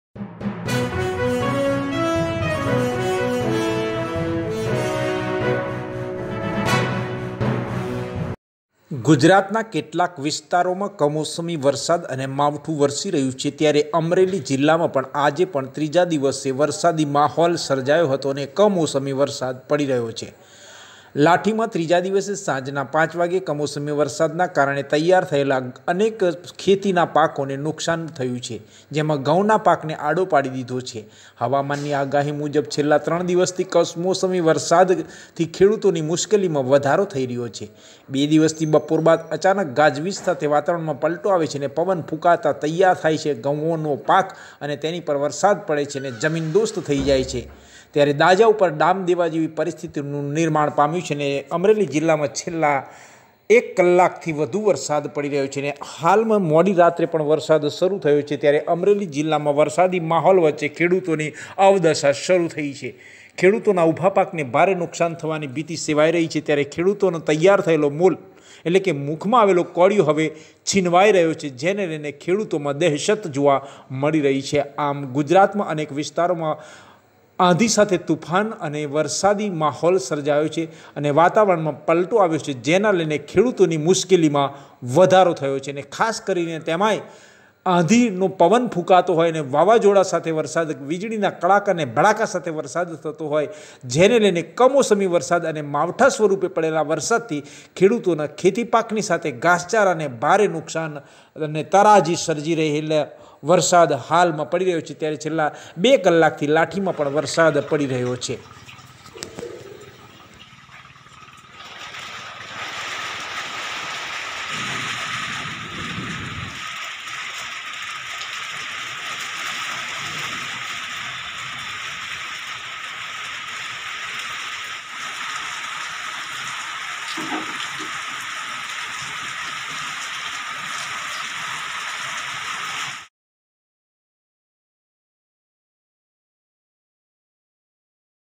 લાઠીમાં ત્રીજા દિવસે કમોસમી વરસાદના કારણે તૈયાર થયેલા ઘઉંના પાકને નુકશાન બે કલાકથી વરસાદ
𝗟𝗮𝘁𝗲𝘀𝘁 𝗡𝗲𝘄𝘀
લાઠીમાં ત્રીજા દિવસે સાંજના પાંચ વાગ્યે કમોસમી વરસાદના કારણે તૈયાર થયેલા ઘઉંના પાકને આડા પાડી દીધા.
       હવામાનની આગાહી મુજબ છેલ્લા ત્રણ દિવસથી કમોસમી વરસાદથી ખેડૂતોમાં મુશ્કેલી વધી છે.
      બે દિવસથી બપોર બાદ અચાનક ગાજવીજ સાથે વાતાવરણમાં પલટો અને પવન ફૂંકાતા તૈયાર થયેલા ઘઉનો પાક જમીન દોસ્ત થતાં દાઝ્યા ઉપર ડામ જેવી પરિસ્થિતીનુ નિમૉણ થયેલ છે.